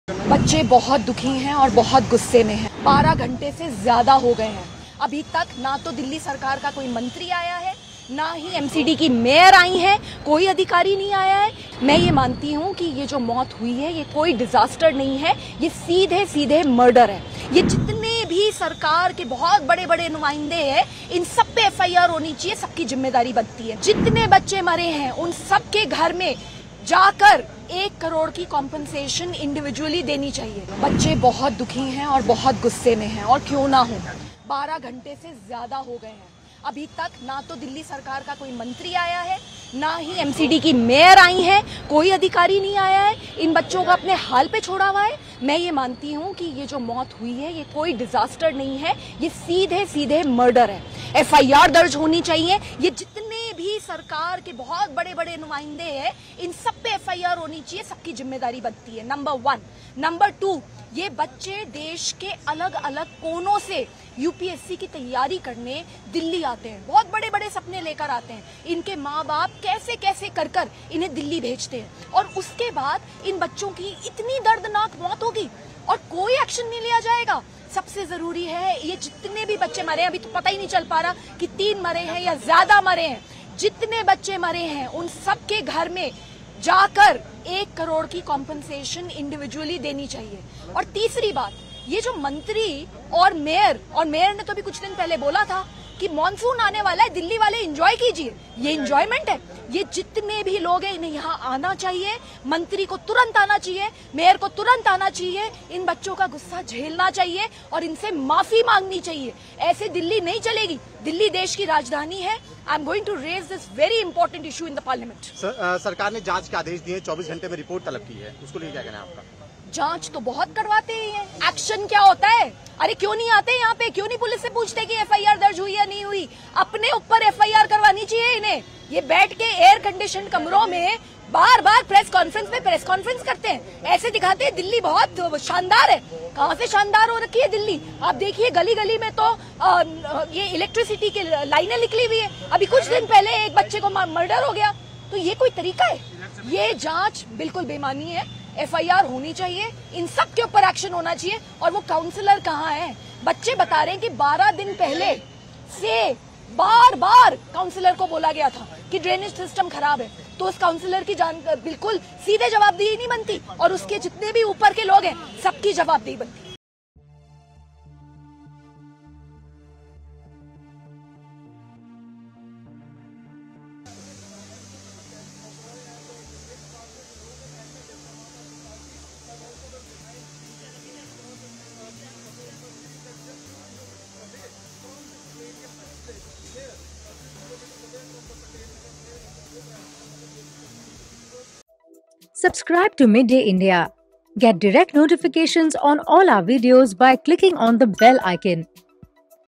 Delhi IAS coaching centre floor incident: Swati Maliwal demands justice for students who died
AAP MP Swati Maliwal has strongly criticized the Delhi government following a tragic incident at the Old Rajinder Nagar Coaching Centre. Speaking to ANI on July 28, Maliwal called for an FIR against senior government officials and demanded a compensation of Rs 1 crore for the families of those who have died. She expressed frustration that no Delhi government minister, mayor, or official has yet visited the site. Maliwal described the deaths as "murder" rather than a disaster and vowed to raise the issue in Parliament. She also highlighted that students had previously alerted a local councillor about issues with the drainage system.